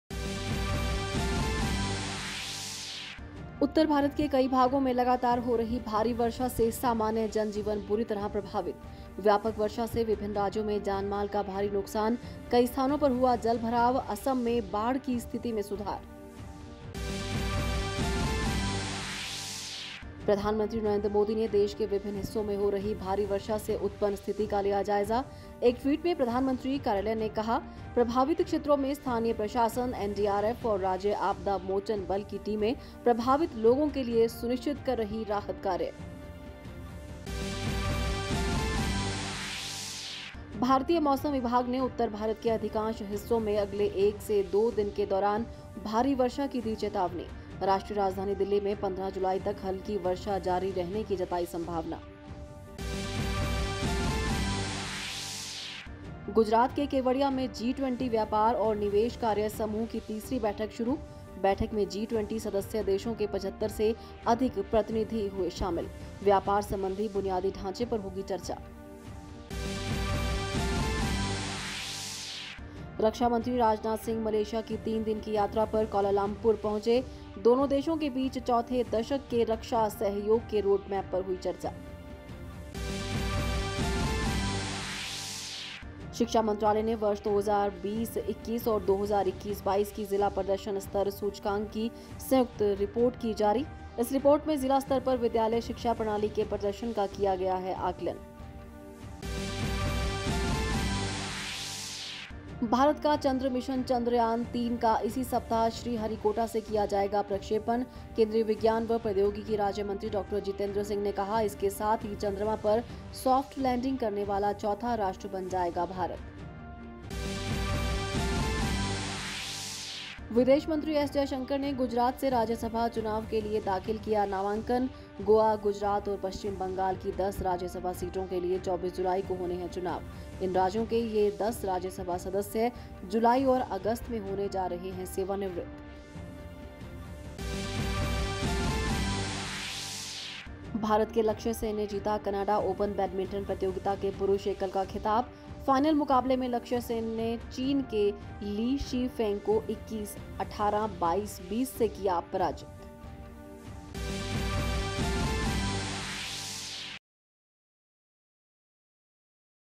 ख़बरें फटाफट।
प्रधानमंत्री नरेन्द्र मोदी ने भारी वर्षा से उत्पन्न स्थिति का लिया जायजा - देखें ख़बरें फटाफट।